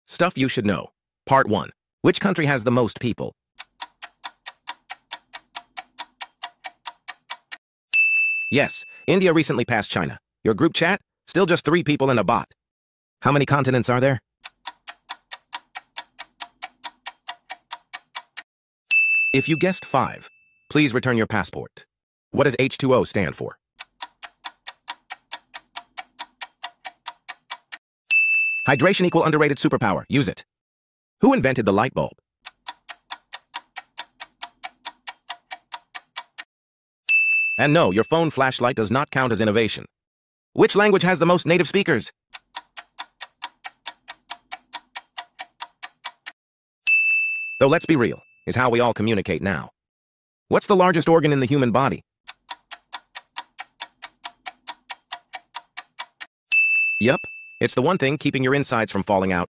What’s the Largest Organ? (No, It’s Not Your Ego 😏)
Think you know the human body? Bet you didn’t see this one coming…
Drop your answer in the comments — and yes, “Ego” is not scientifically accepted. Yet.